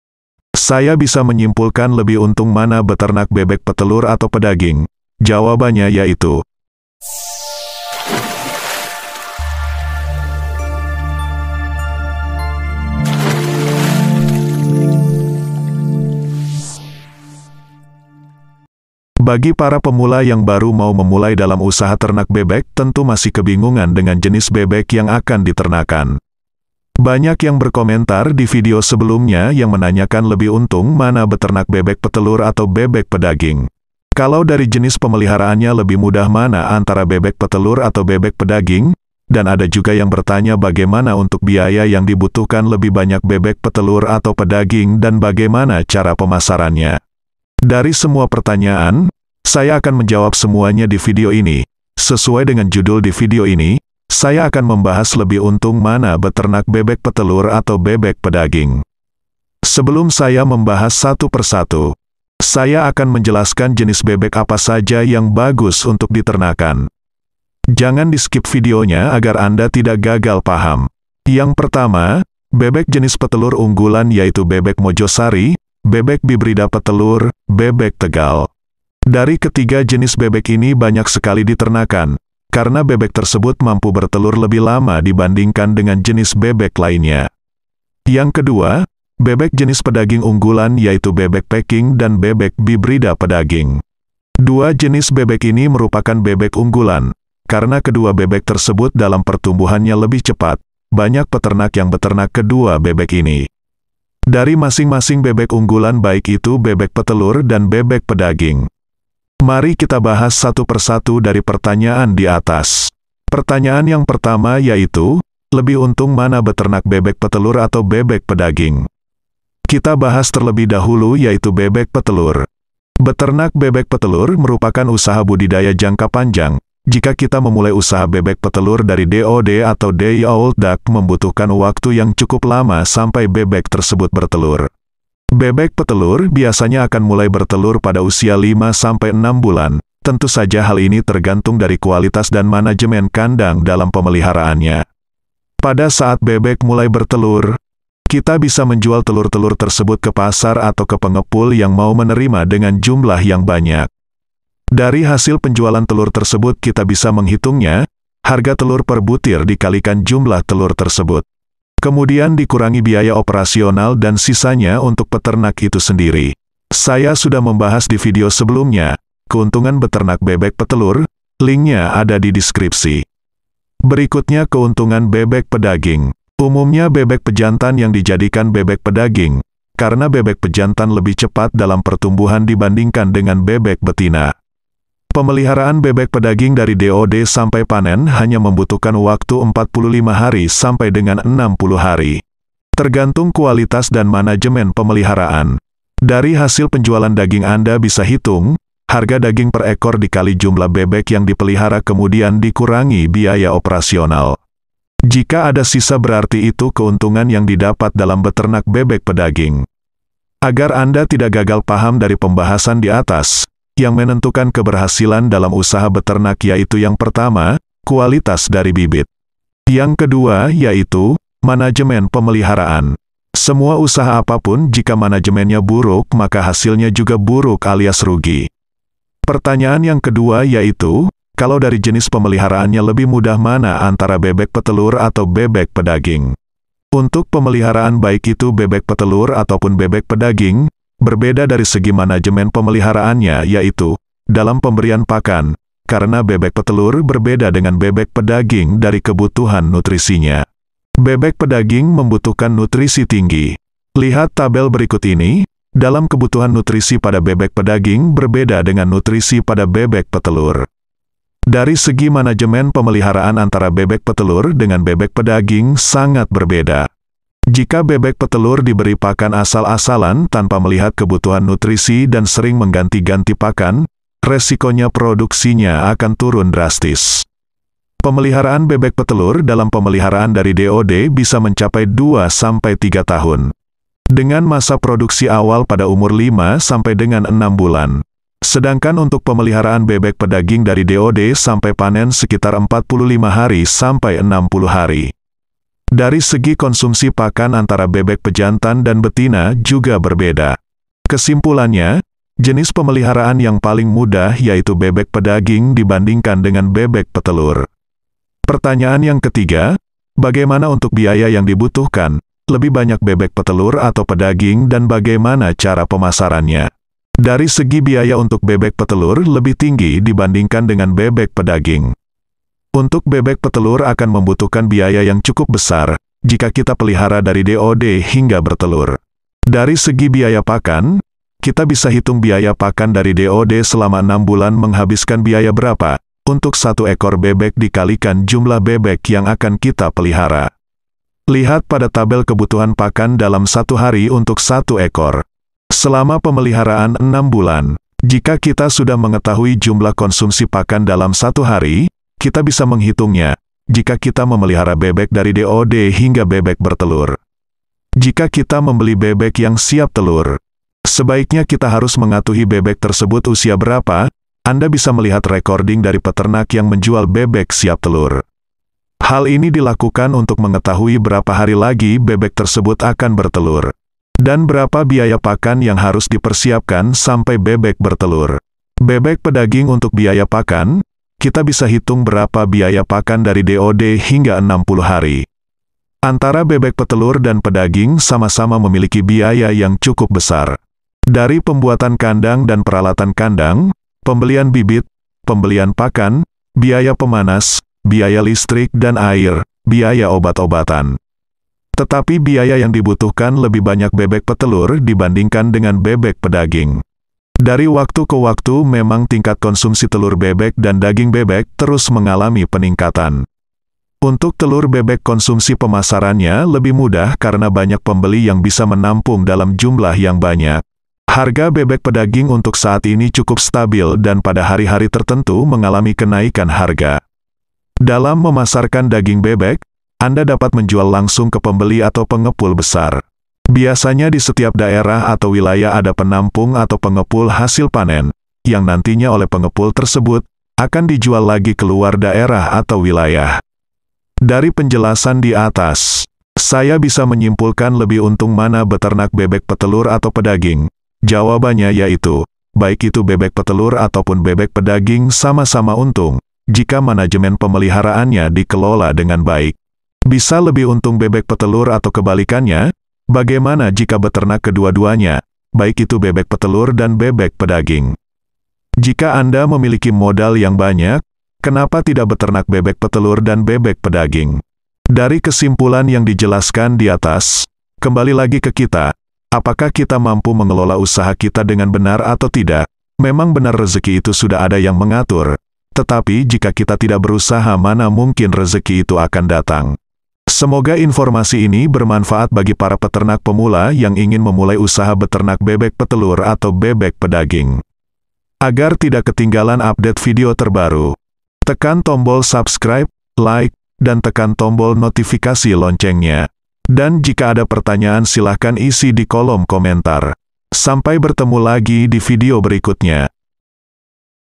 Lebih Untung Mana Ternak Bebek Petelur Atau Bebek Pedaging
Lebih untung mana ternak bebek petelur atau bebek pedaging. Dalam ternak bebek atau itik, baik itu bebek petelur ataupun bebek pedaging, merupakan usaha ternak yang sama-sama menguntungkan. Jika manajemen pemeliharan yang baik, bioscurity yang ketat, maka akan menghasilkan pundi pundi uang atau cuan yang banyak. Anda bisa memilih jenis bebek yang ingin diternakan seperti bebek mojosari, bebek peking, bebek rambon, bebek tegal, bebek hibrida. Di kesempatan ini saya akan memberikan informasi keuntungan dari masing-masing usaha tersebut. Anda bisa memilih salah satu atau keduanya untuk membudidayakan ternak bebek petelur atau pedaging. Anda bisa memilih menggunakan sistem kandang baterai bebek petelur ataupun kandang panggung, atau jika anda ingin beternak bebek pedaging dengan sistem kandang panggung atau postal. Jika ada pertanyaan, silahkan isi di kolom komentar.